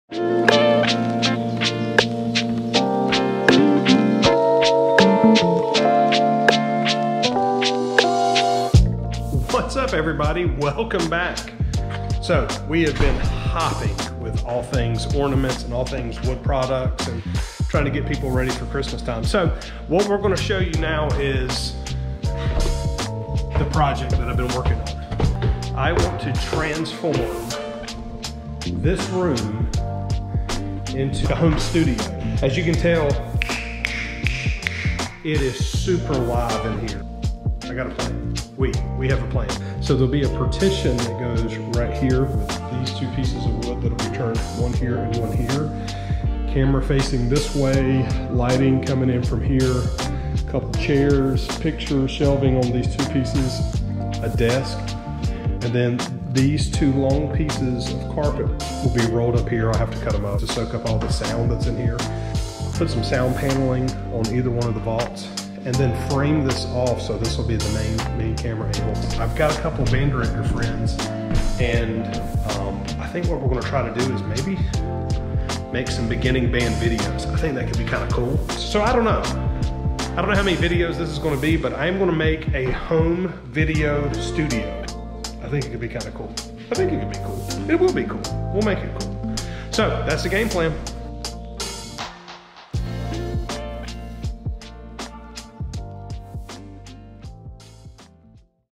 Making a Home Video Studio out of my bonus room!
Making a home video studio out of the bonus room.